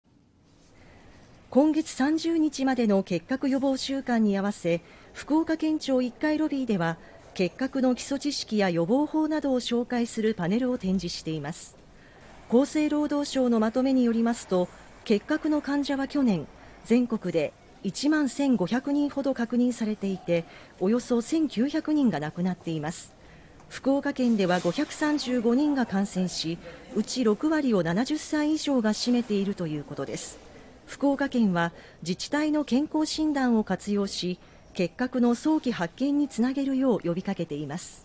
今月２４～３０日は「結核予防週間」 福岡県庁のロビー展で正しい知識を
「結核予防週間」にあわせて正しい知識を身につけてもらおうと福岡県庁でパネル展が開かれています。

今月３０日までの「結核予防週間」にあわせ福岡県庁１階ロビーでは、結核の基礎知識や予防法などを紹介するパネルを展示しています。

厚生労働省のまとめによりますと結核の患者は、去年全国で１万１５００人ほど確認されていて、約１９００人が亡くなっています。

福岡県では５３５人が感染し、うち６割を７０歳以上が占めているということです。

福岡県は、自治体の健康診断を活用し結核の早期発見につなげるよう呼びかけています。